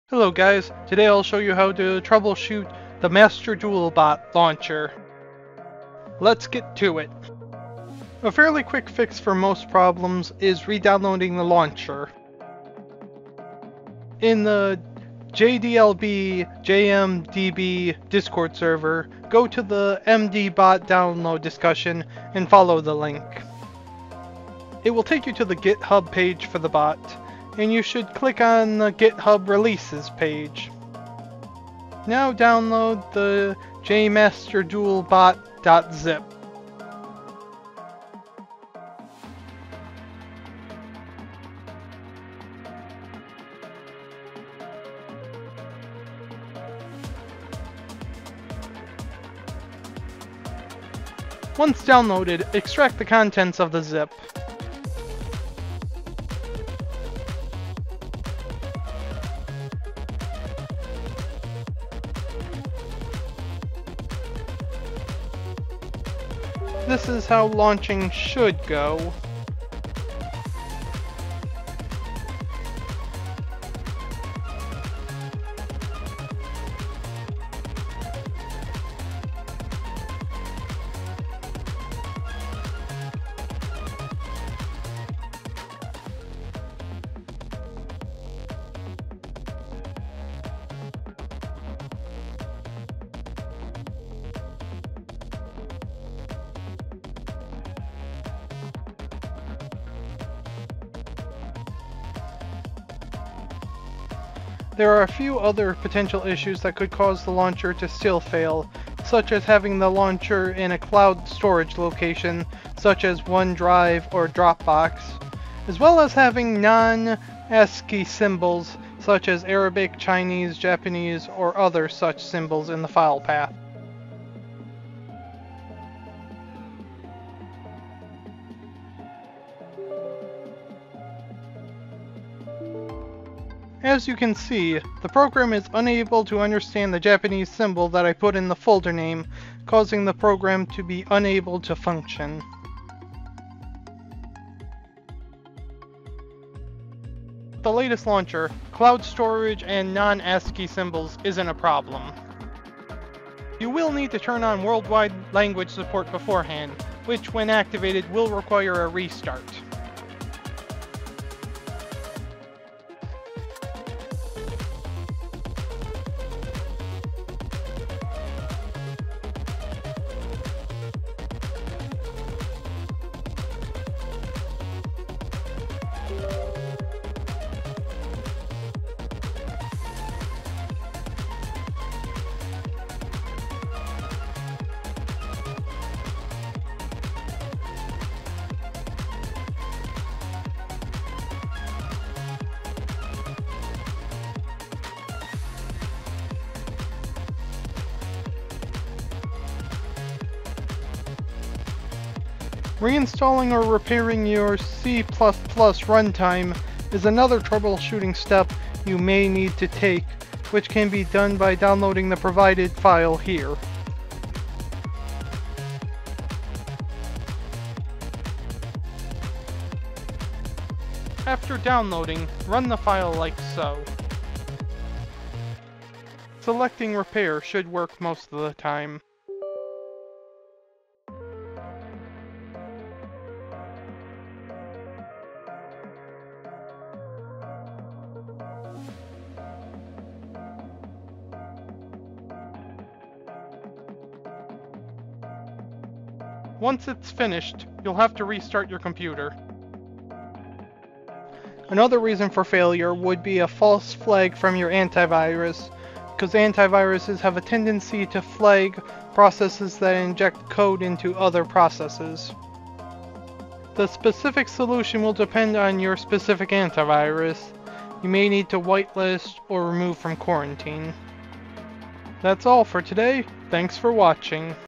Troubleshooting JMaster Duel Bot/JDuel Links Bot Launcher not working 💯👍
In this video you'll see how to troubleshoot the JMaster Duel Bot and/or JDuel Links Bot launchers. Some people reported that the application didn't open. This is usually caused by an outdated C++ Runtime or a false Antivirus detection.